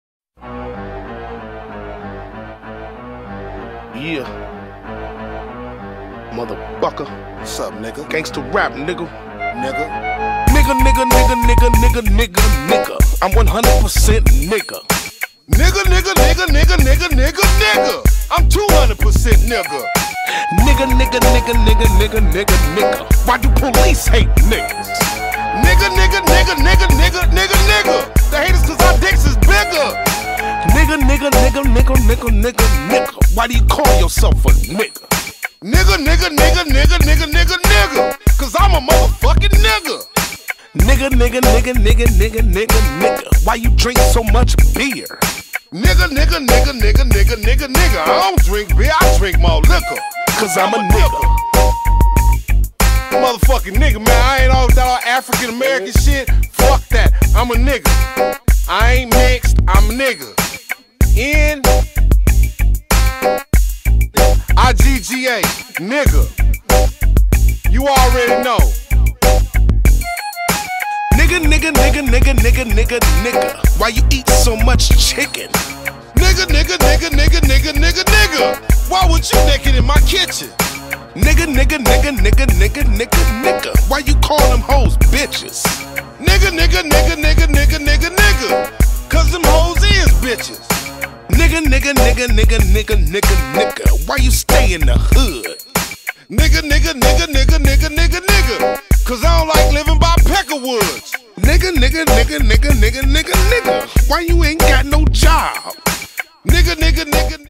David Guetta Goes FULL Ghetto with Afrojack & Nicky Romero (LIVE @ Tomorrowland 2013)
David Guetta, Afrojack & Nicky Romero dropping Gangsta Rap at Tommorowland :)